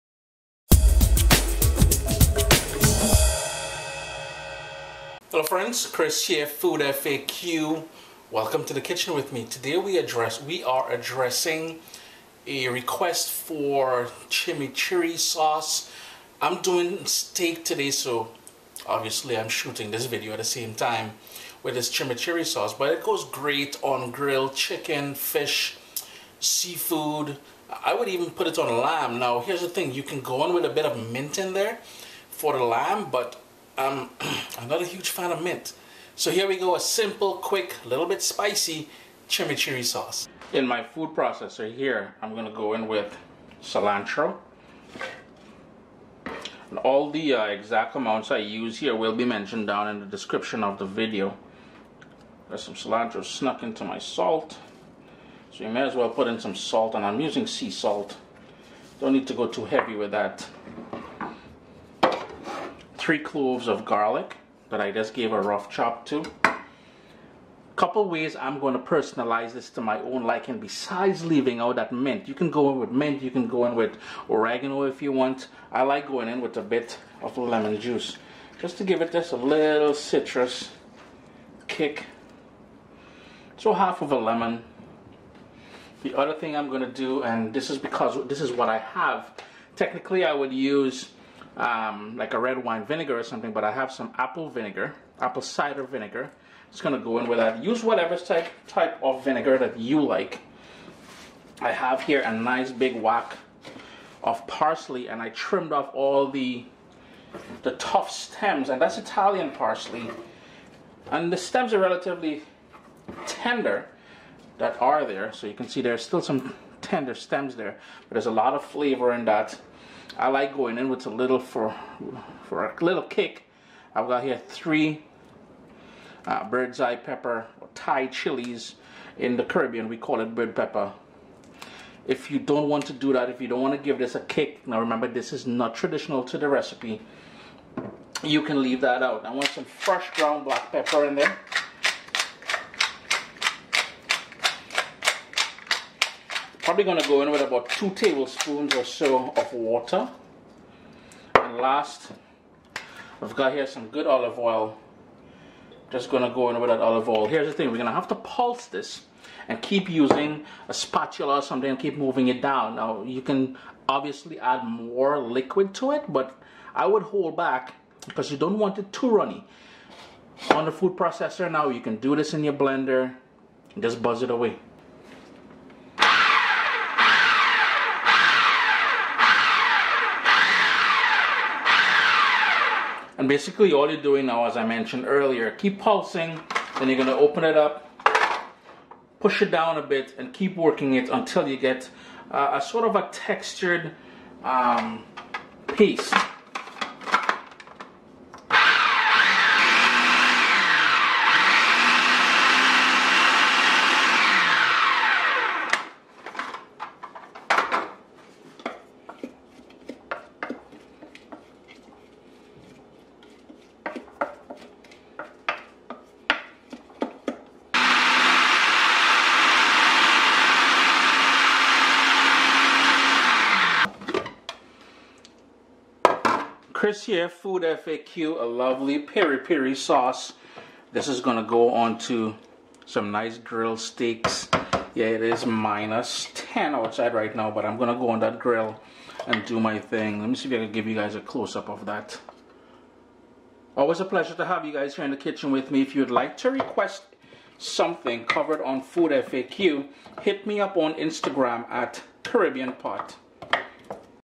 Amazing Chimichurri Sauce FoodFAQ | ChrisDeLaRosa.com
Learn how to make a tasty Chimichurri sauce with simple step by step instructions from Chris De La Rosa. Chimichurri sauce is and excellent topping/condiment for steaks (beef), chicken, pork, fish, seafood or grilled vegetables.

For this chimichurri sauce you'll need..

3 cloves garlic
1/2 lemon
1/4 teaspoon sea salt
large bunch Italian Parsley (abt 1-2 cups loosely)
small bunch cilantro (about 1 cup - loosely)
3 bird's eye pepper
2 tablespoon apple cider vinegar
2 tablespoon olive oil

Get my Gourmand Award winning cookbook, The Vibrant Caribbean Pot - 100 Traditional And Fusion Recipes Vol 2 @